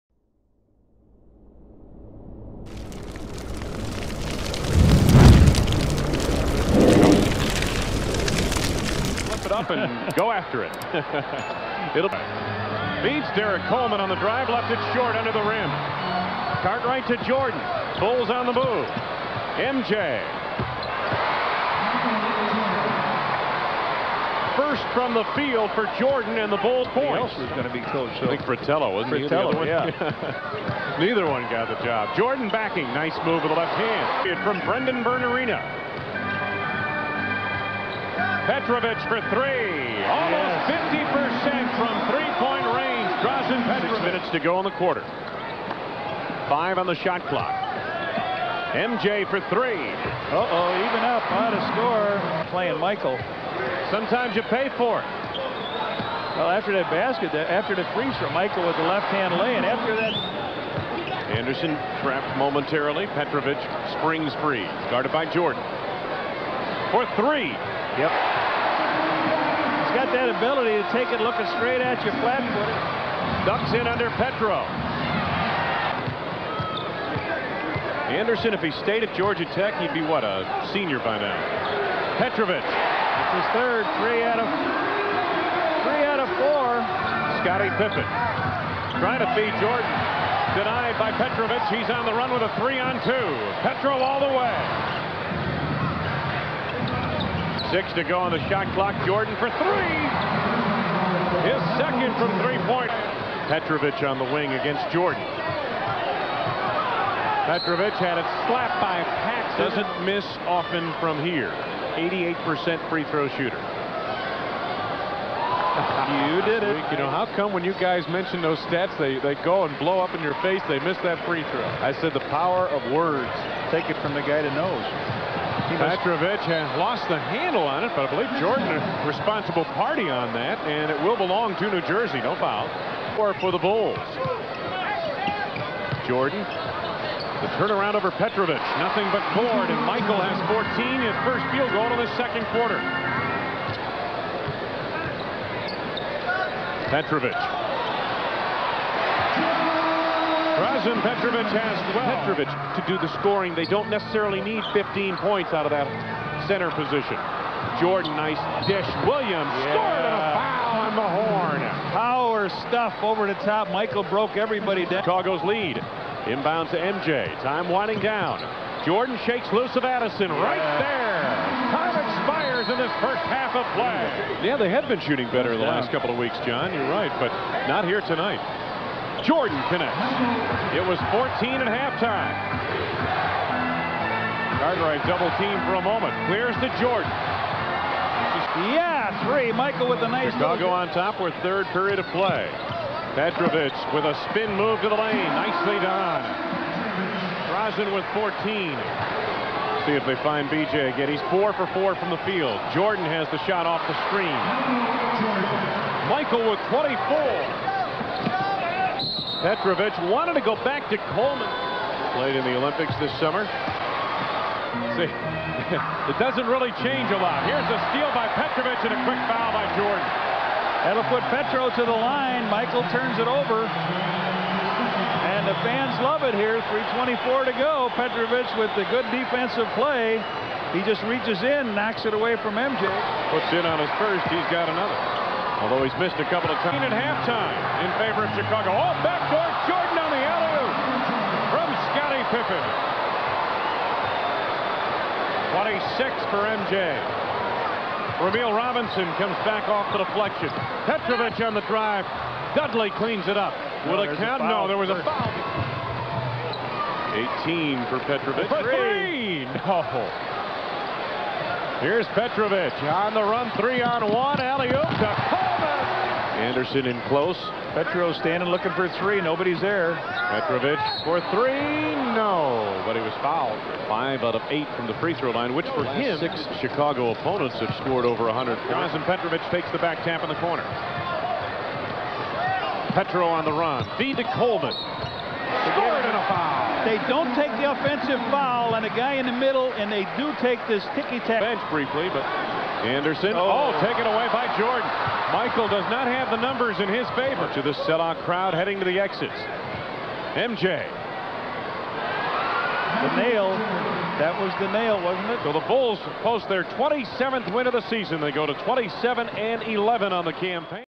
1 ON 1 | Drazen Petrovic (20 pts) VS Michael Jordan (30 pts) | + DRAZEN's: IN YOUR FACE JORDAN!
GREAT 1 ON 1 DUEL